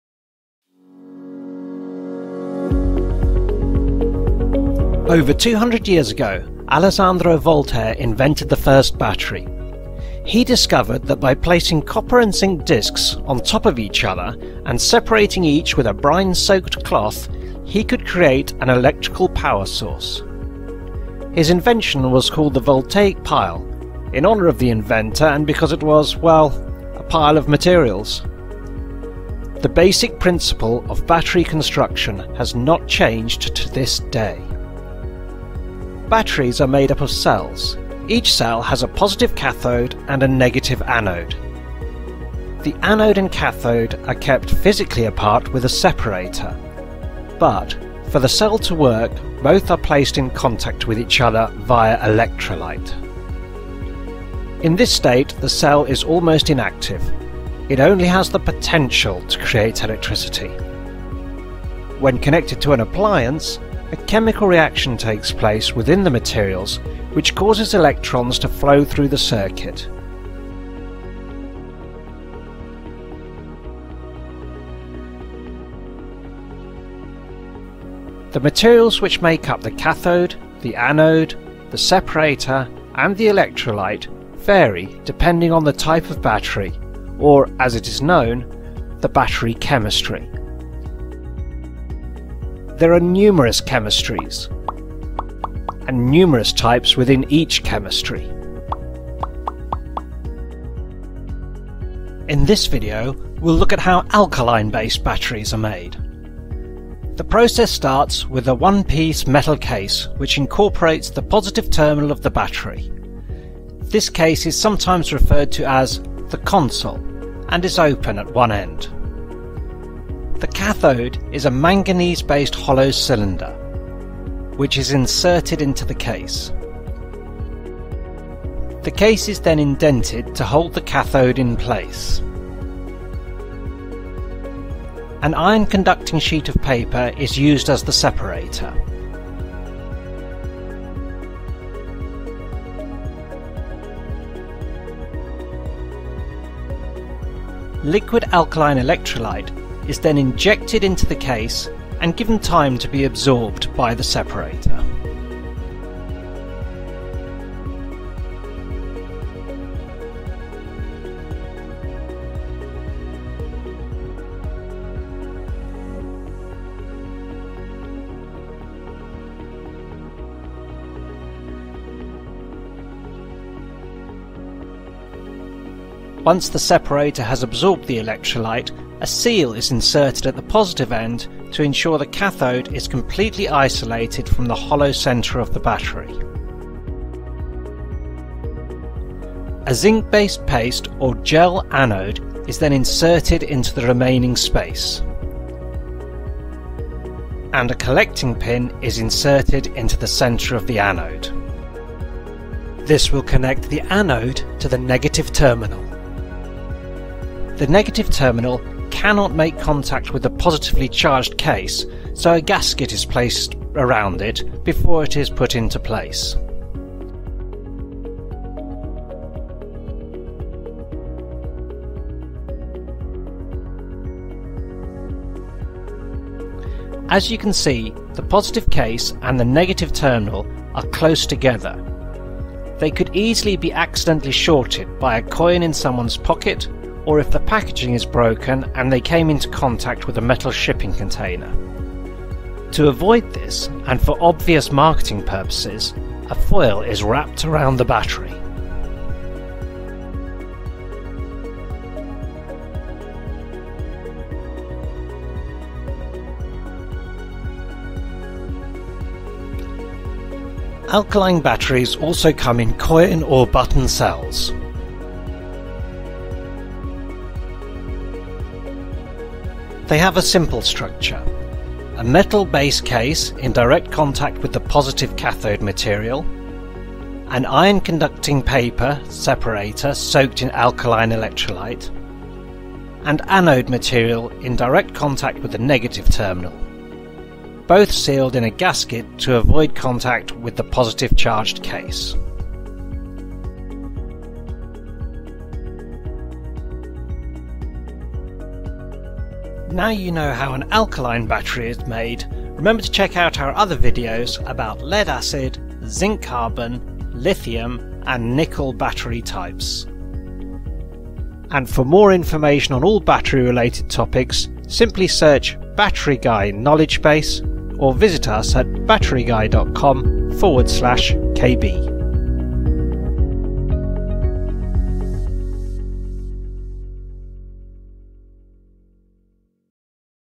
How Alkaline Based Batteries are Made
To find out more about the world of batteries and portable power visit our Knowledge Base

In this film we'll show you the process that goes into making an Alkaline based Battery.